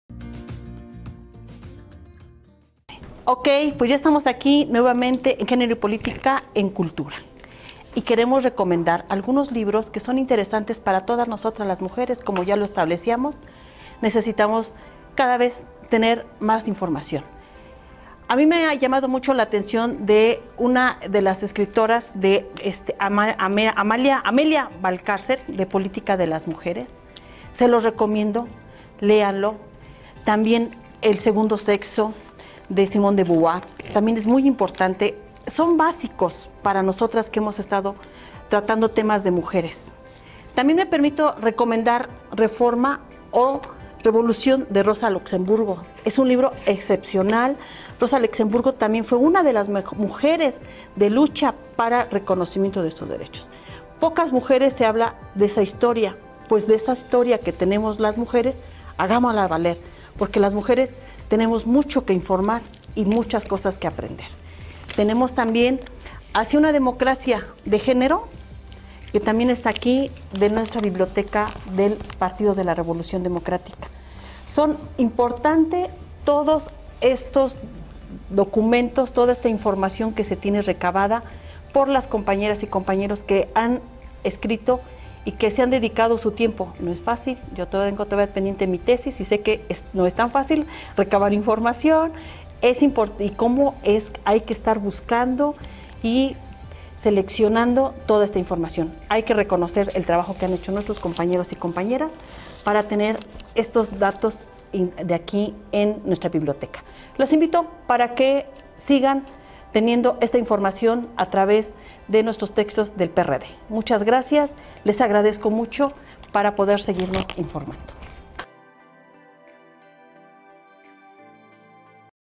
CULTURA POLÍTICA CON ERNESTINA RAMIREZ
En esta sección Ernestina Ramirez recomienda el libro La política de las Mujeres de Amelia Valcarcel, El segundo sexo de Simone de Beauvoir. Ademas de textos que el PRD ha editado.